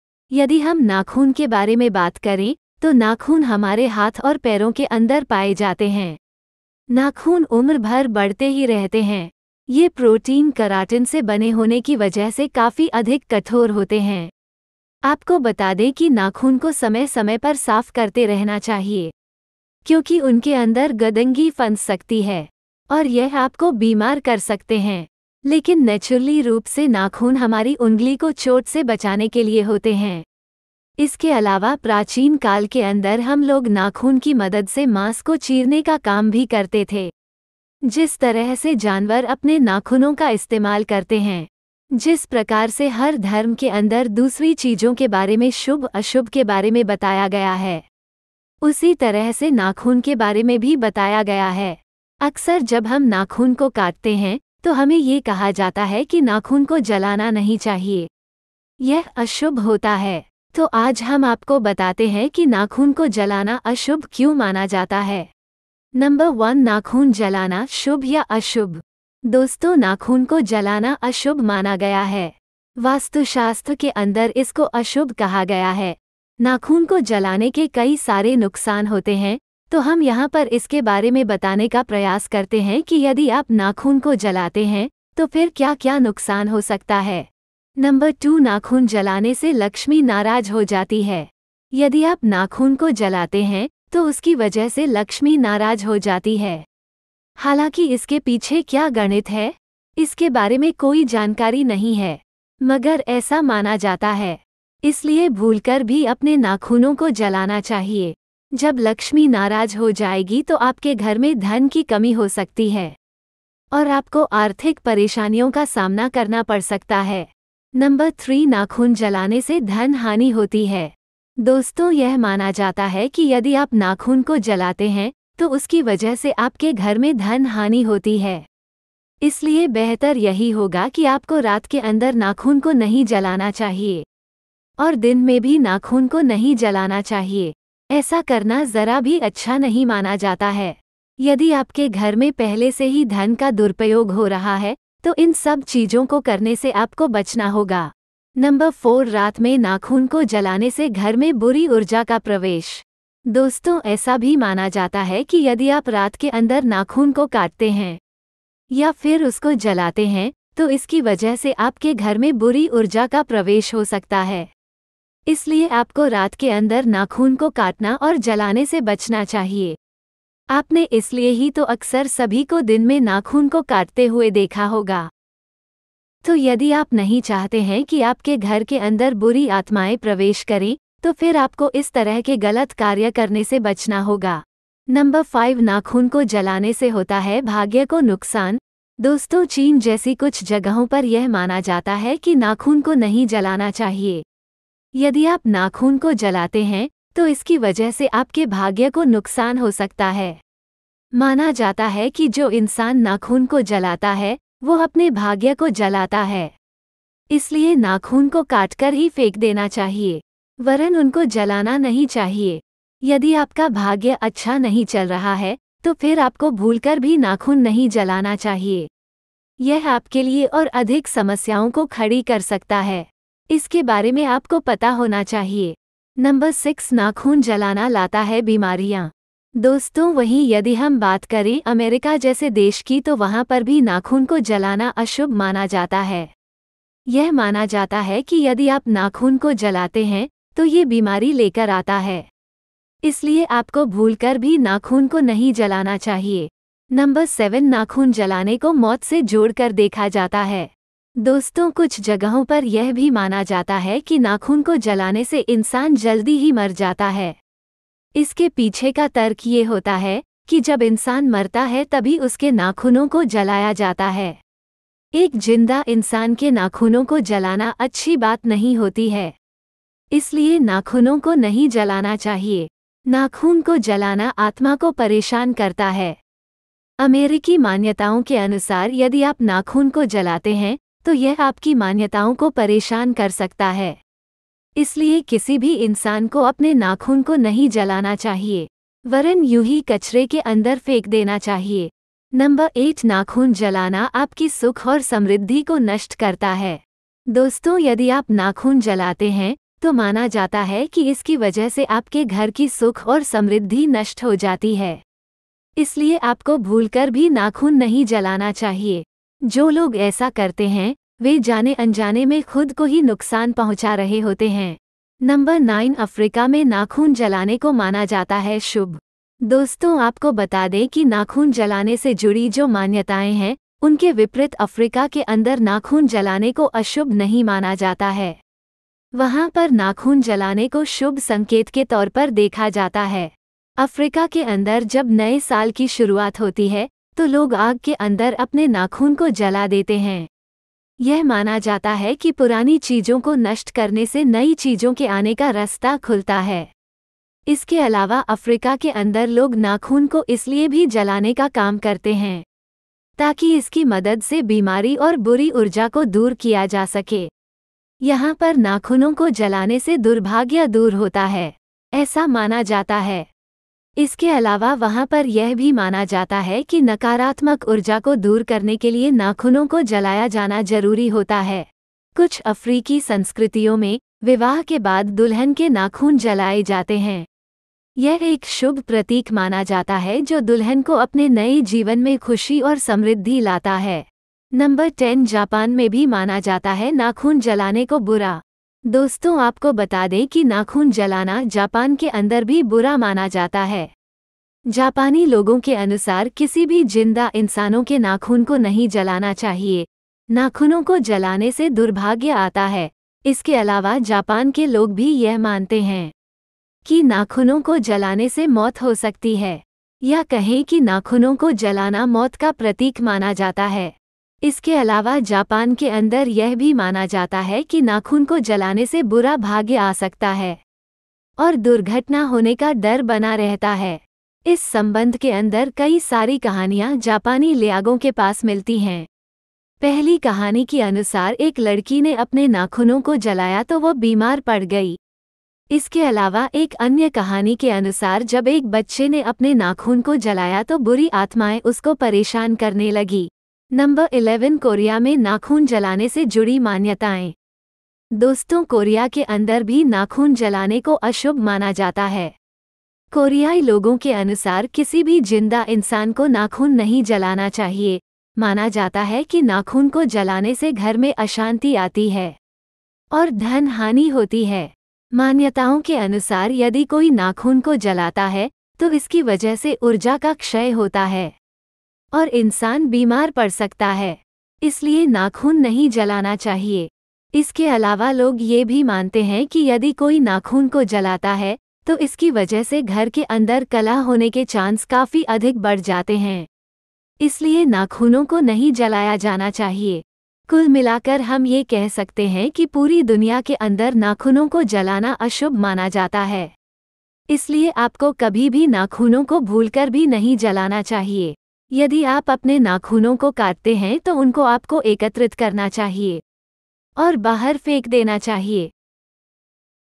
नाखून काटकर जलाने से क्या होता है/ nakhun katkar jalane ke fayde
नाखून काटकर जलाने से क्या होता है nakhun katkar jalane ke fayde  नाखून जलाने के कई सारे भयंकर नुकसान होते हैं। तो इस विडियो को पूरा देखें और कमेंट करके बताएं ।